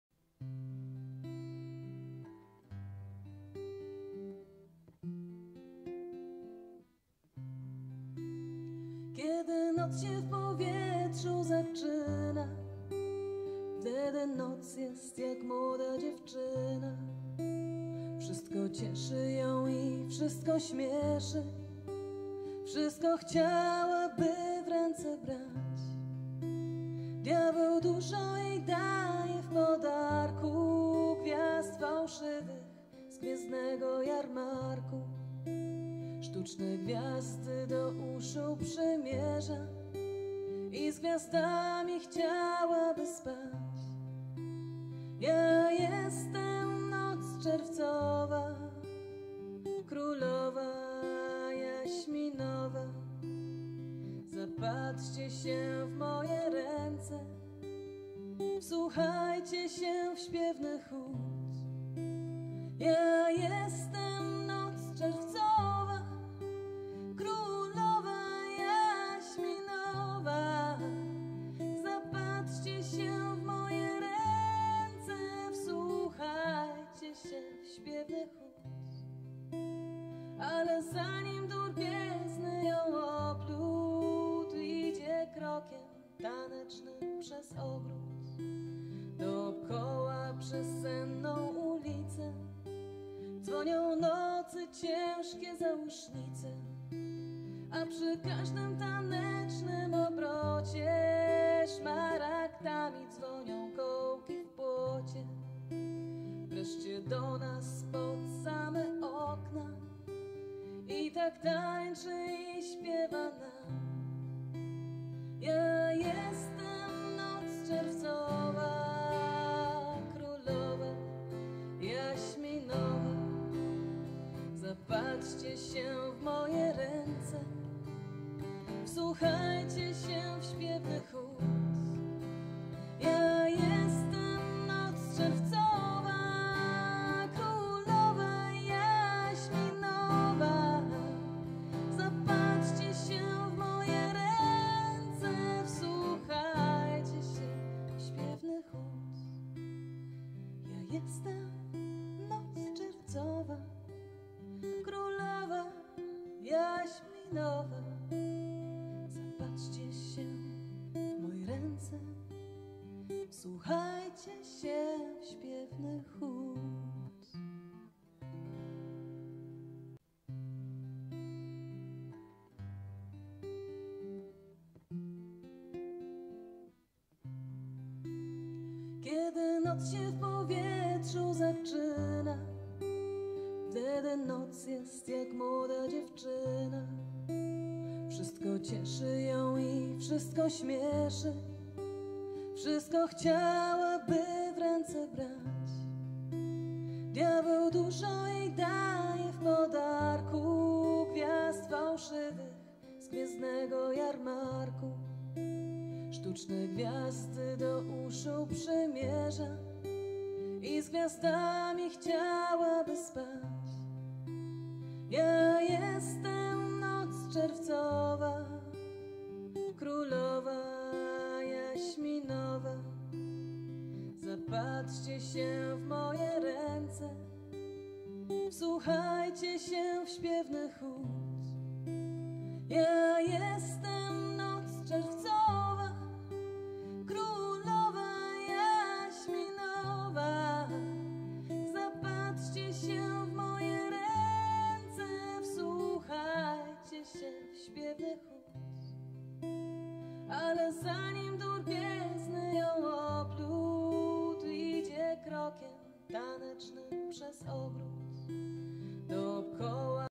Świętojanki 2020
IX Poetycka Noc Świętojańska 2020 w studiu pepe-tv, wyjątkowo, z powodu pandemii, online.